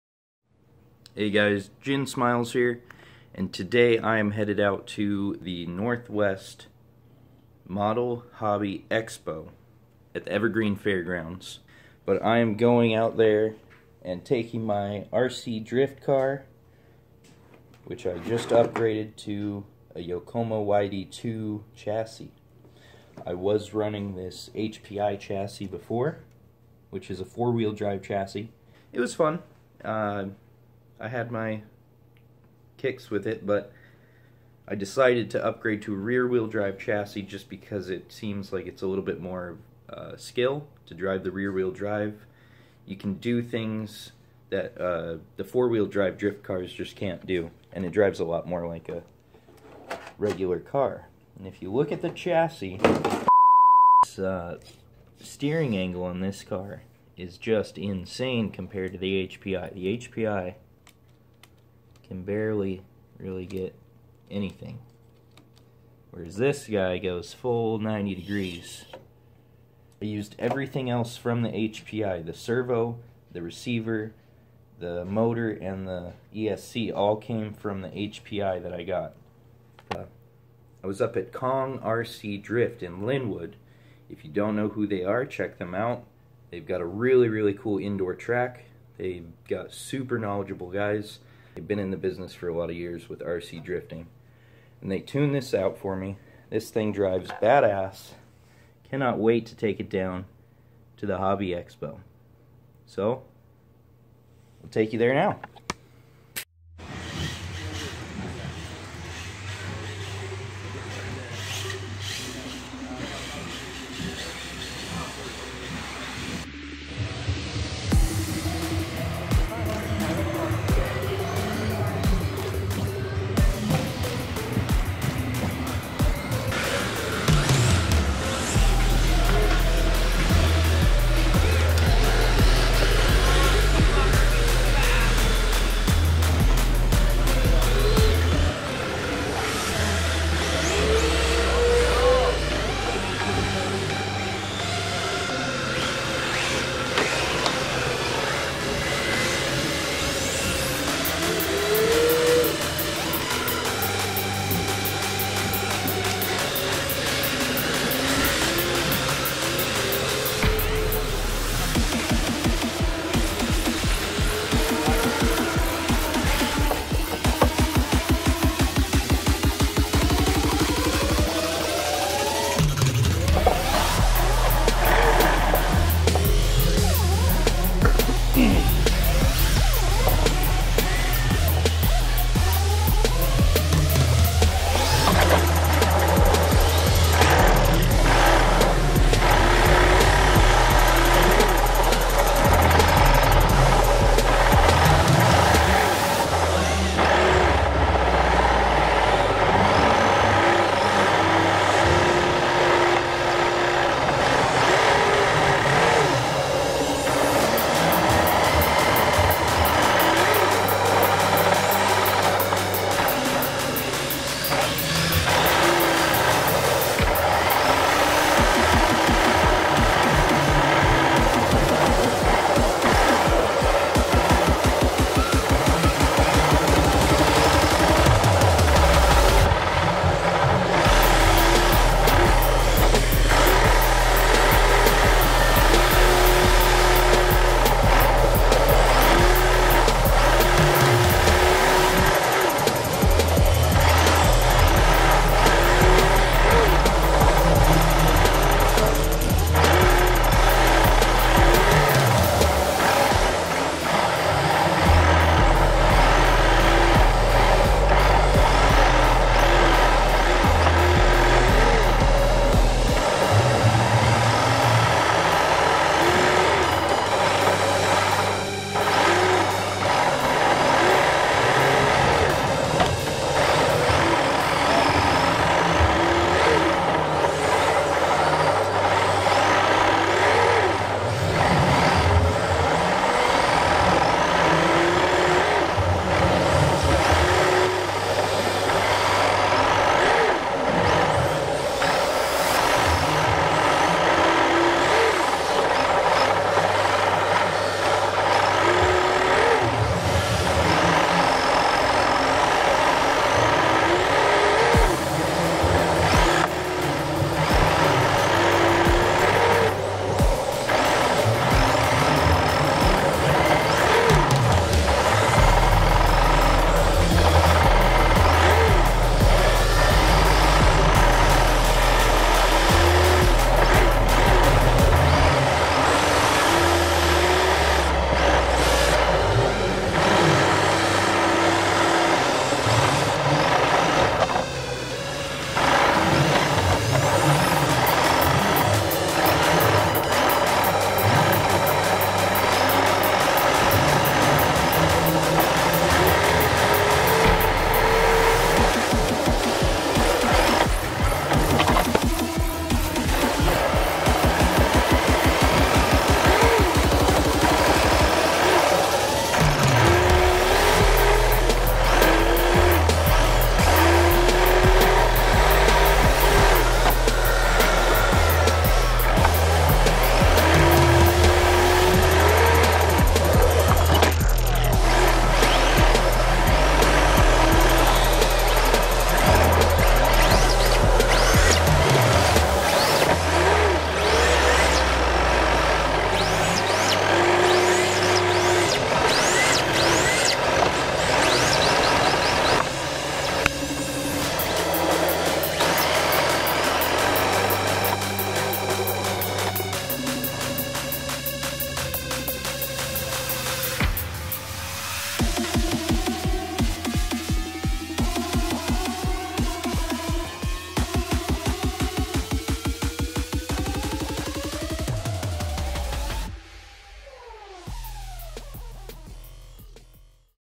(RC Drift) NW Model Hobby Expo - Friday
Day 1 of the NW Model Hobby Expo at the Evergreen Fairgrounds in Monroe WA. Sorry for the crappy FPV will get better shots as the weekend continues!